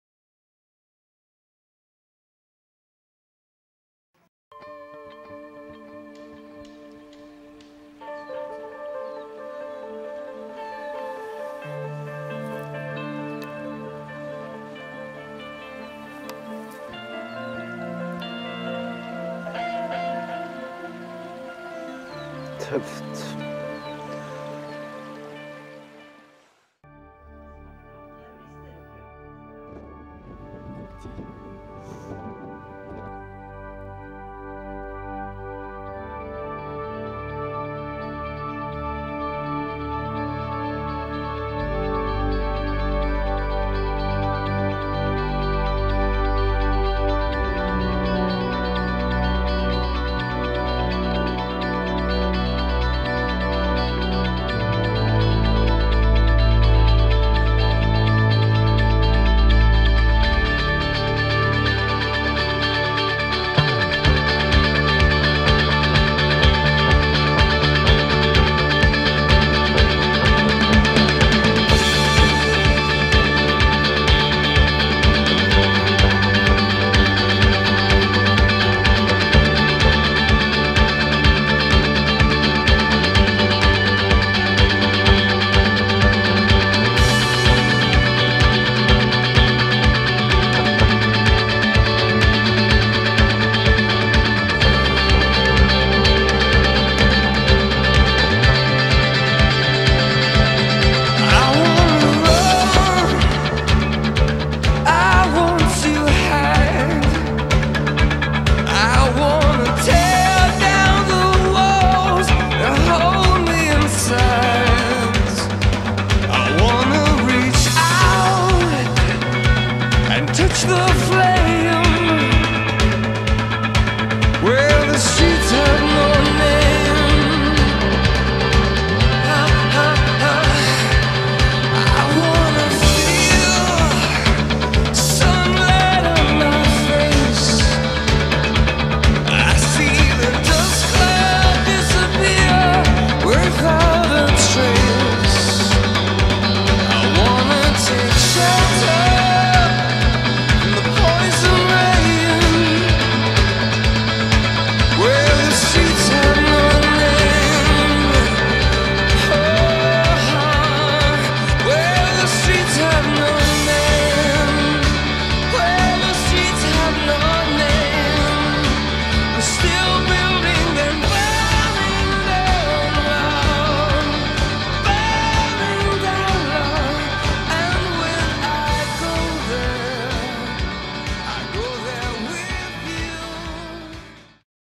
Fly Fishing New-Zealand
Fant et gammelt klipp fra NZ i 2006, litt dårlig kvalitet men..Old clip from NZ 2006.